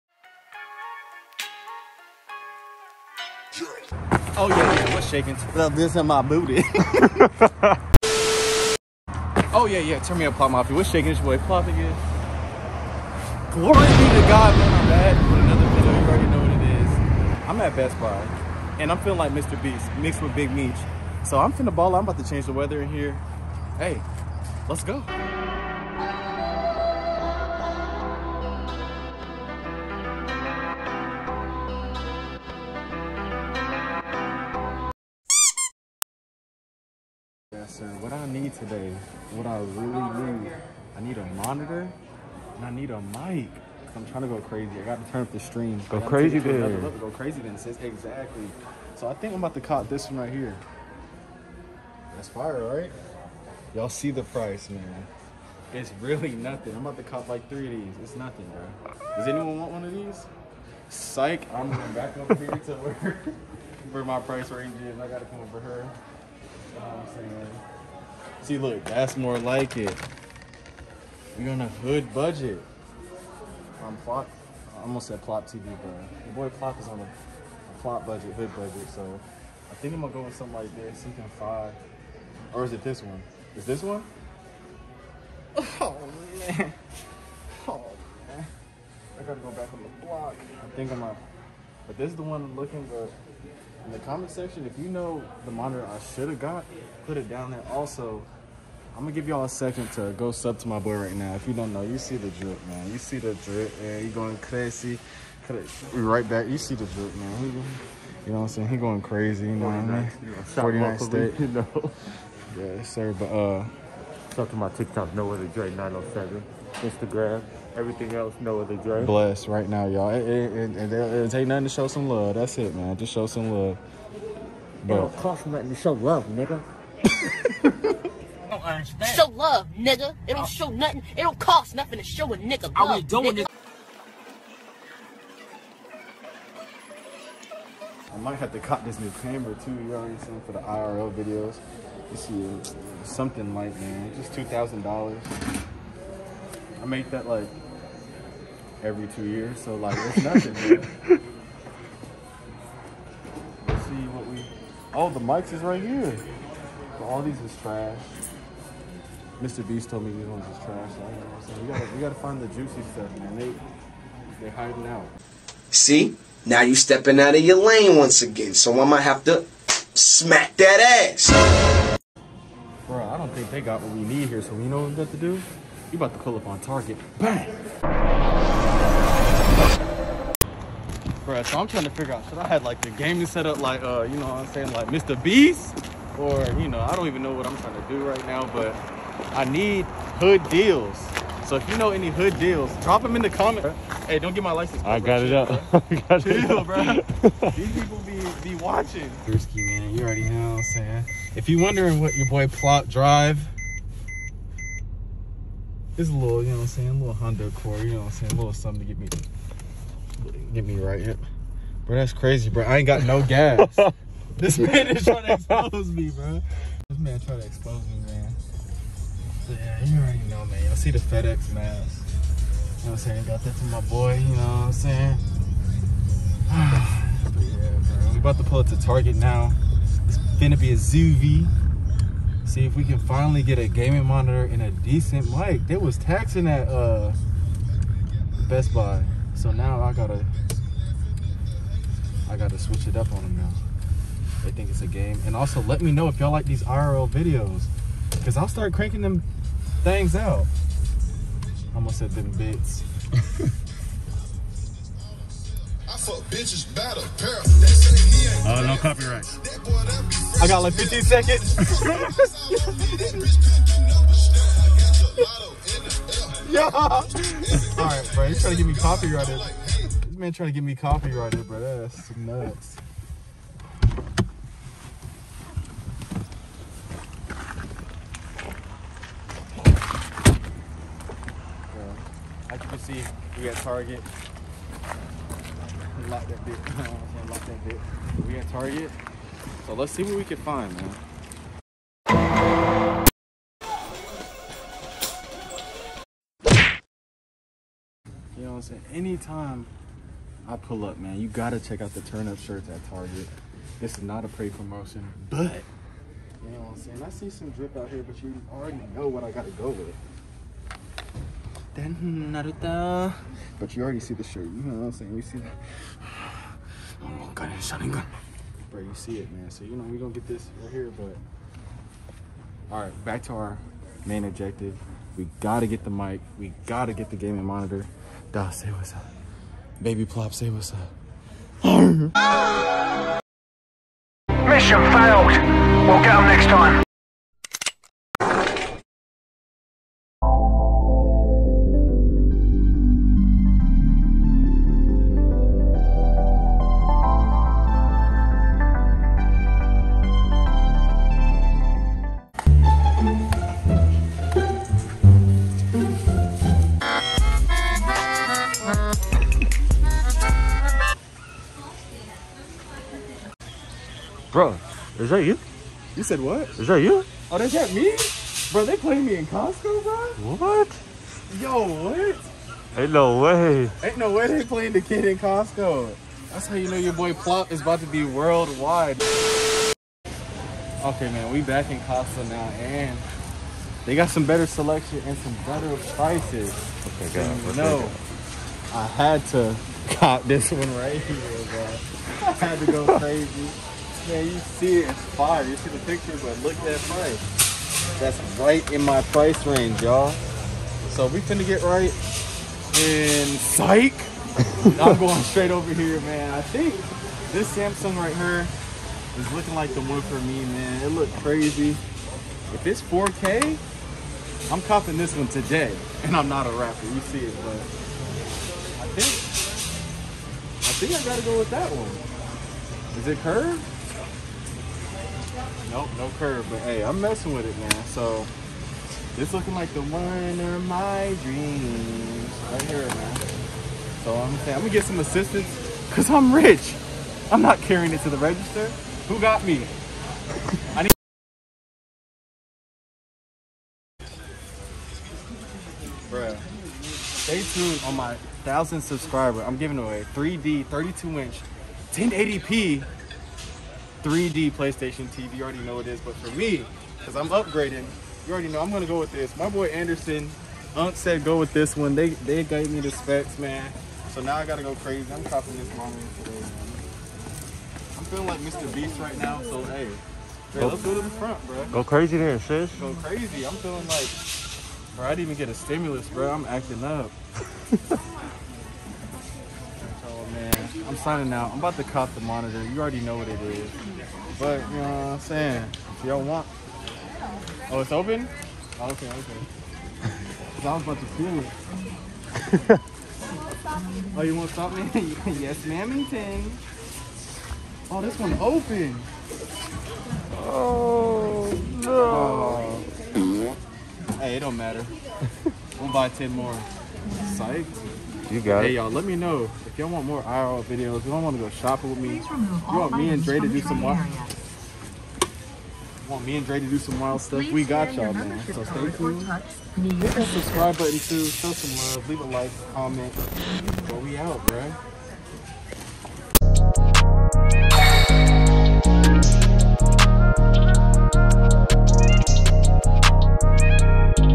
shopping for gaming set up vlog | best buy, target, costco
Shopping vlog searching for the best budget gaming monitor!
Link for the product purchased

As always BIG Shoutout to PLOP MAFIA